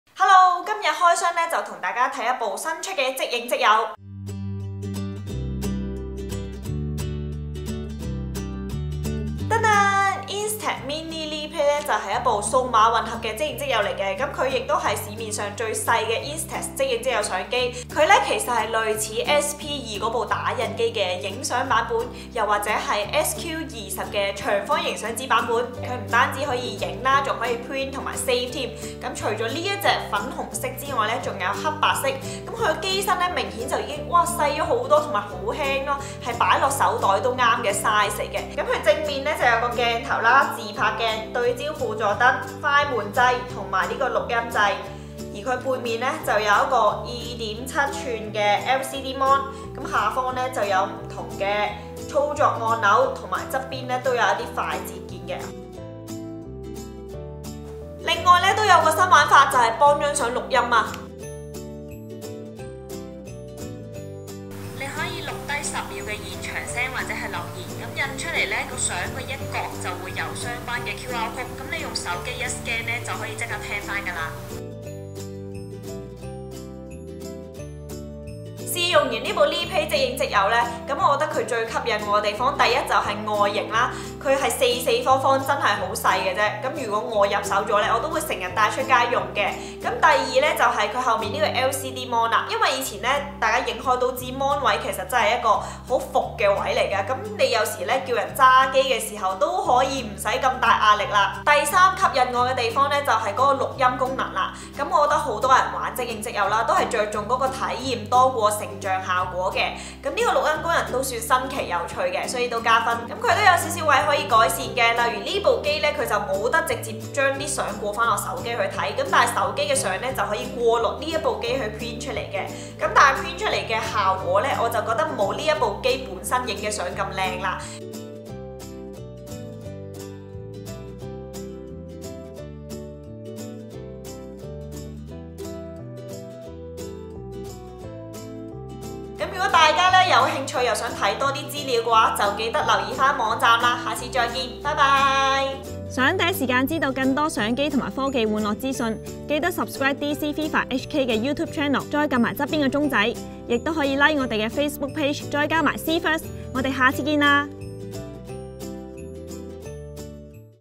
最輕巧即影即有 新增10秒錄音功能 Fujifilm Instax Mini LiPlay
Fujifilm Instax Mini LiPlay 一部可以做到「影相、Print相、Save相」，仲可以錄返個留言！外形夠輕巧放入手袋仔都冇問題，機背加埋個LCD顯示屏，影相芒位即時準返好多～